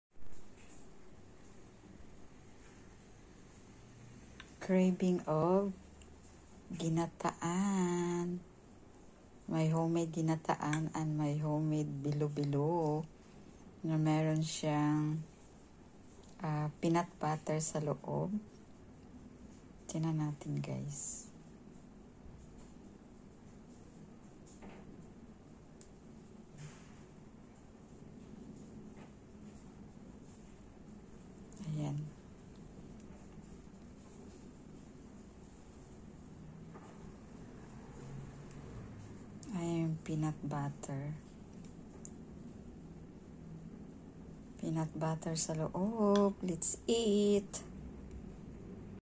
Craving of Ginataan with homemade BILO-BILO
Home made Bilo -Bilo or Ginataan let's eat yummy 😋